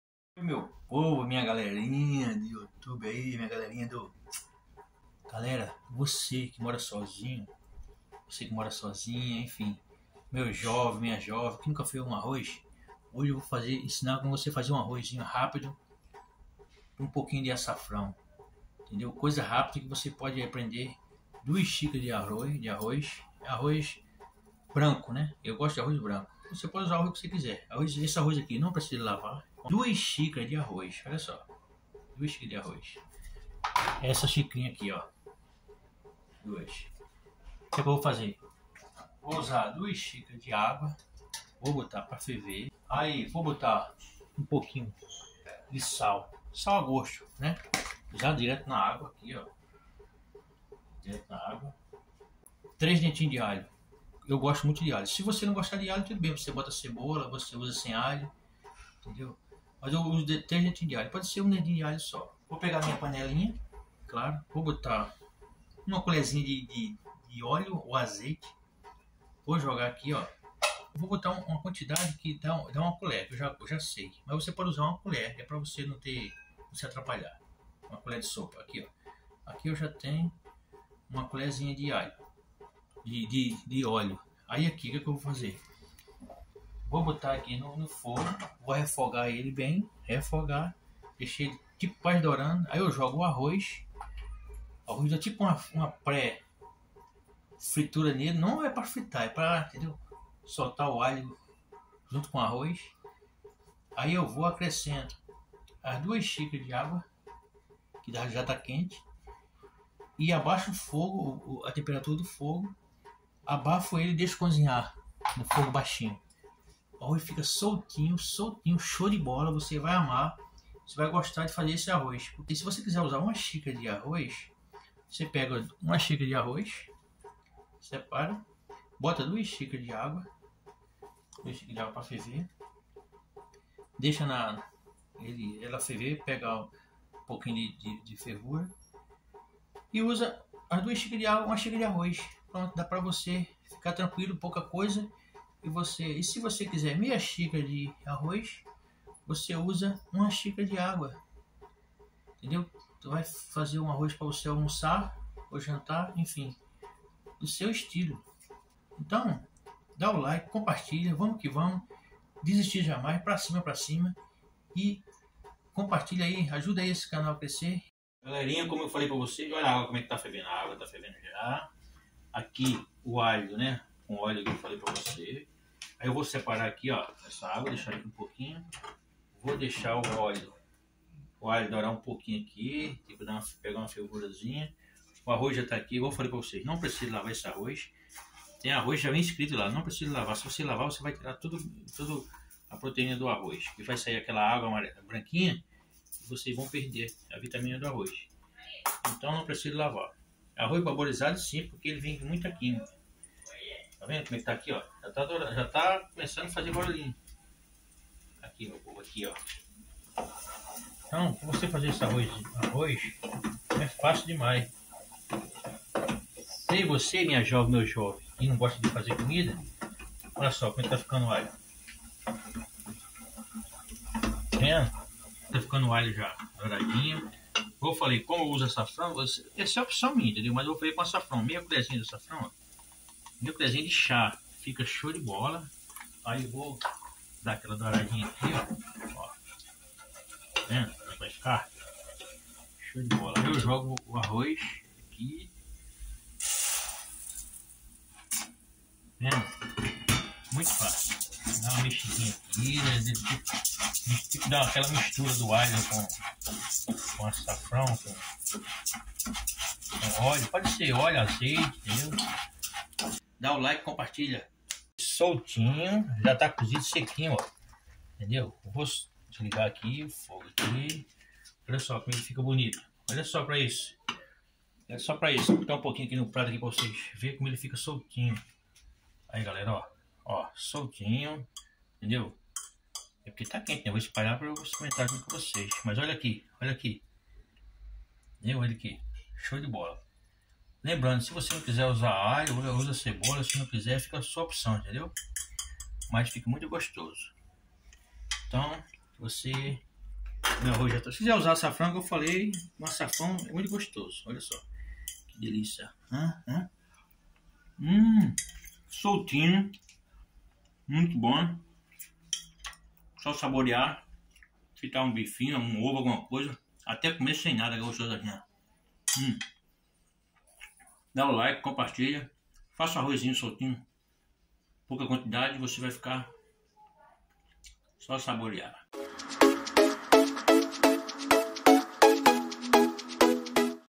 Arroz com Açafrão
Vem com o Vital aprender a fazer arroz com açafrão. Fica muito bom!
Vocês vão amar!!!

Se gostou do vídeo cliquem em GOSTEI, logo acima da descrição!

Nem melhor, nem pior, apenas diferente. Até o próximo vídeo. Abraço!!!